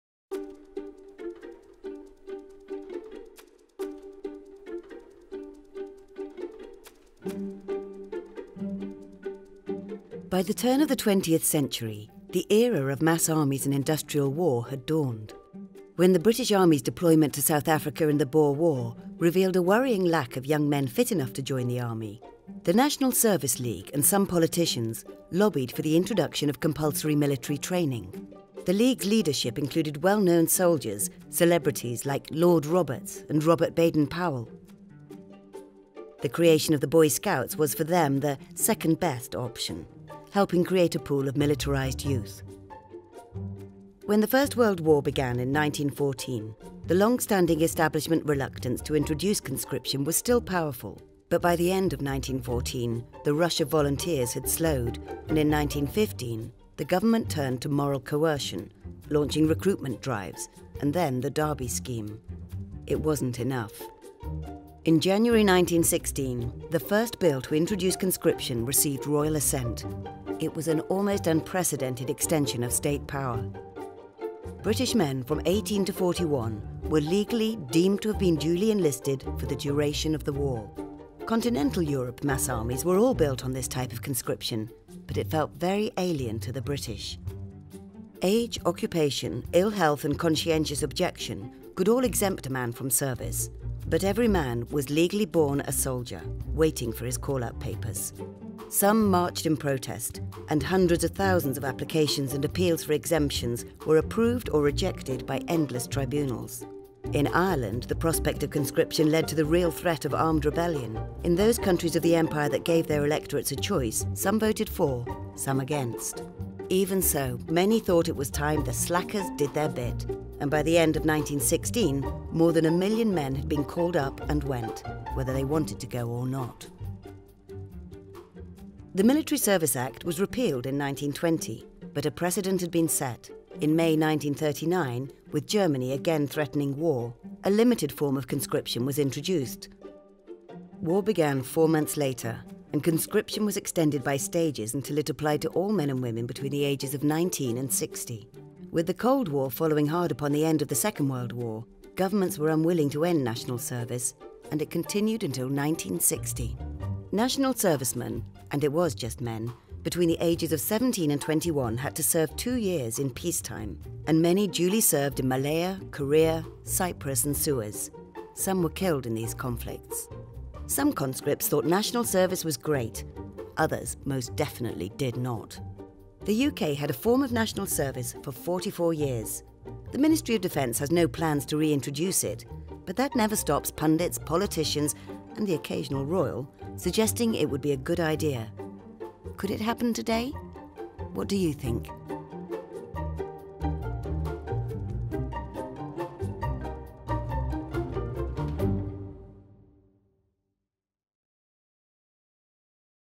The Story of Conscription
By the end of 1915, the number of volunteers for military service was dwindling. In March 1916, after much debate, the UK government changed the law. Outside Ireland, all UK citizens were now soldiers, precluded from call-up only by gender, age, physical fitness or occupation.

Compulsory military service of one form or another continued until 1960.

This video explores the pre-war origins and the story of conscription using objects from the National Army Museum’s collection.